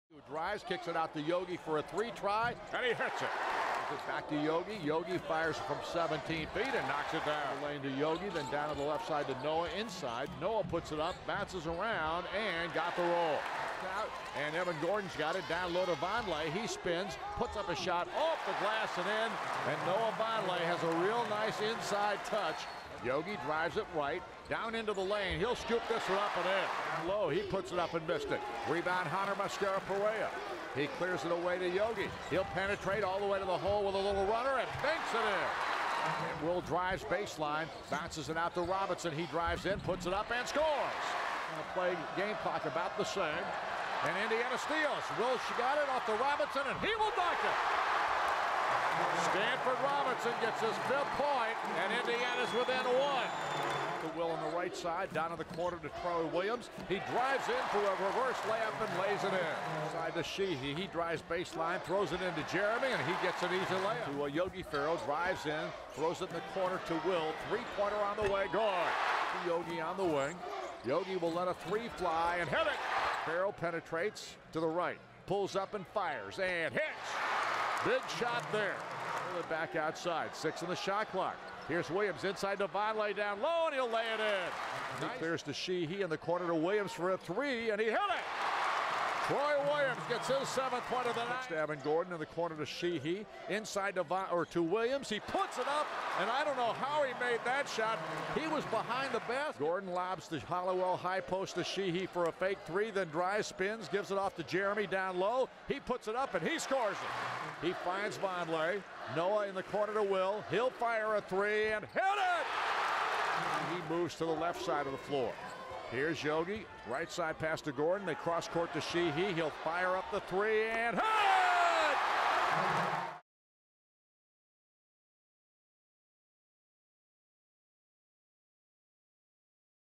LIU Brooklyn at Indiana Game Highlights
Senior Will Sheehey scored 15 of his 19 points in the second half, making the go-ahead 3-pointer with 1:47 left to lead Indiana past pesky LIU-Brooklyn, 73-72, on Tuesday, Nov. 12, in Assembly Hall.